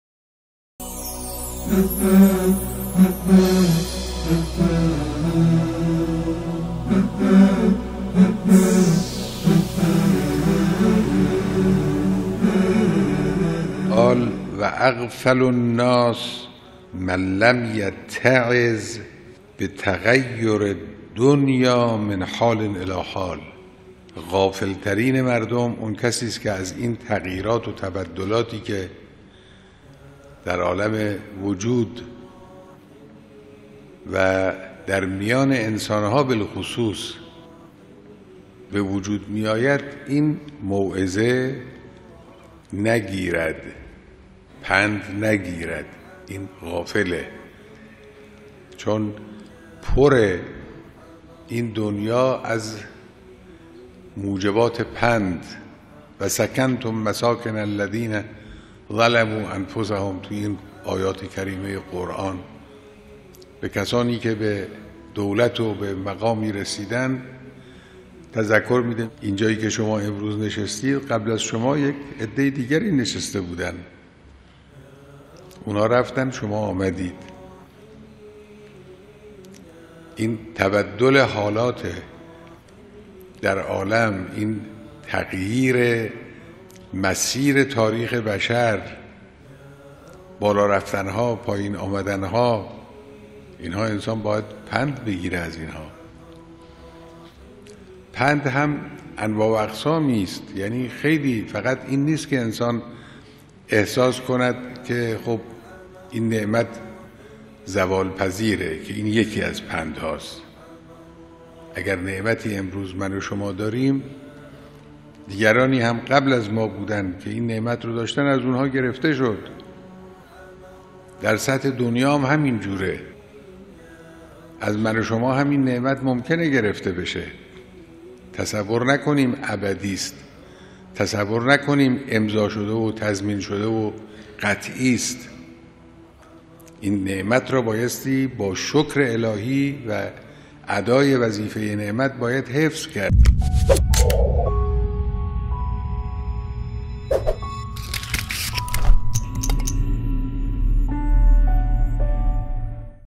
The Most Negligent Person
Who is the best of people in your eyes? The most generous ones? the most self-giving ones? Or … . Do you like to know who the best of people in the eyes of God is? Watch this clip and get the answer from the tongue of prophet Muhammad(S).